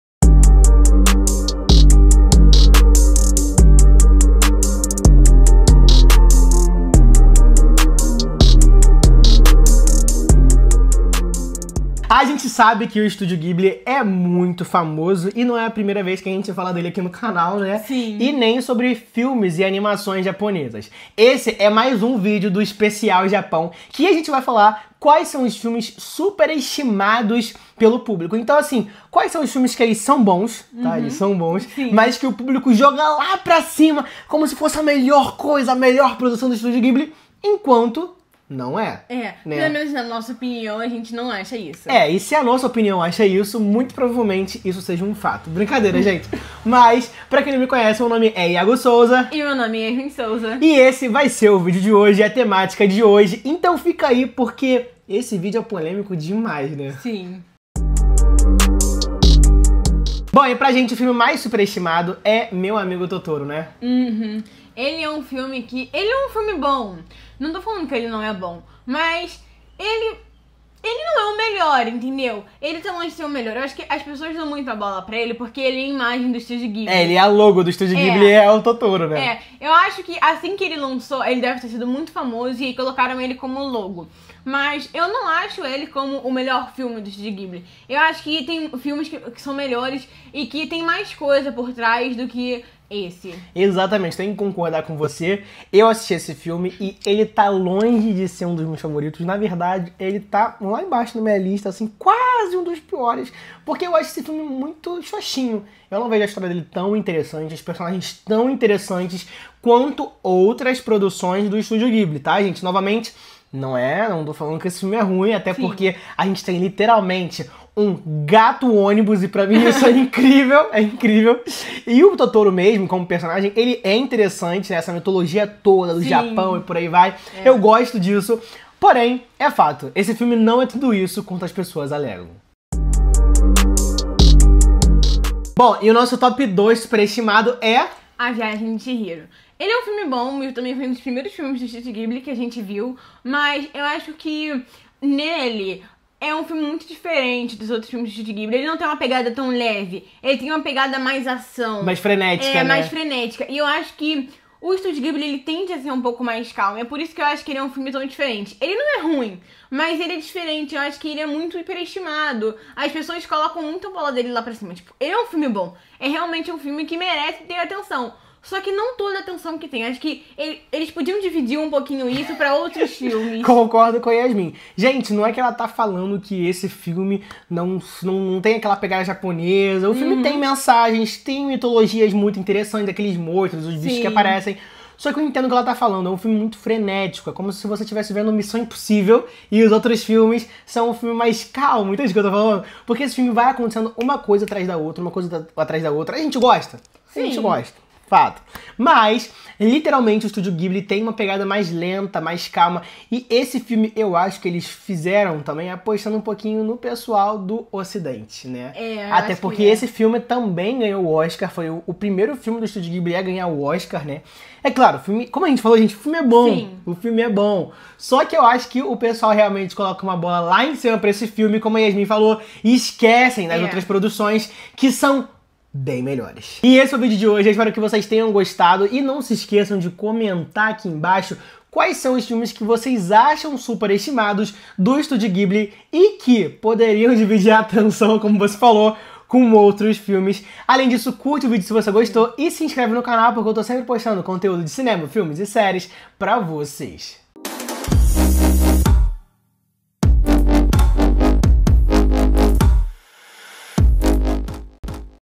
SUPERESTIMADOS do Studio Ghibli! A viagem de Chihiro e Meu Amigo Totoro…
Prepare-se para uma jornada pelo universo mágico do anime e do cinema japonês! Nesta semana especial, vamos explorar os mundos de Studio Ghibli, reviver a emoção de Cavaleiros do Zodíaco e relembrar os clássicos que marcaram gerações. Do anime mais recente ao Godzilla, não vai faltar aventura!